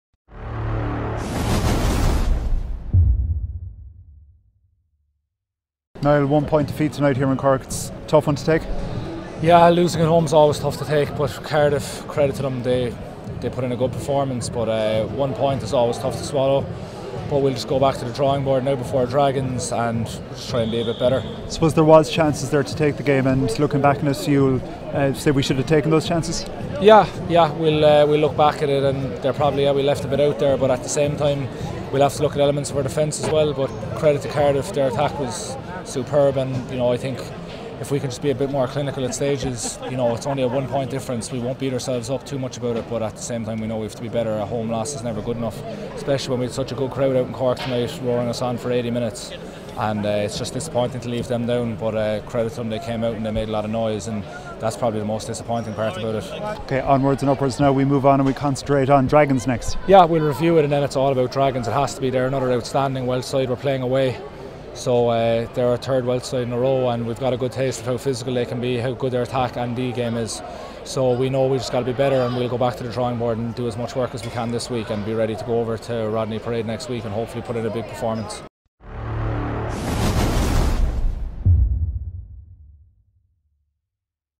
Scannell - Munster v Cardiff Post Match Reaction
Niall Scannell reflects on tonight's narrow loss and looks ahead to Dragons next weekend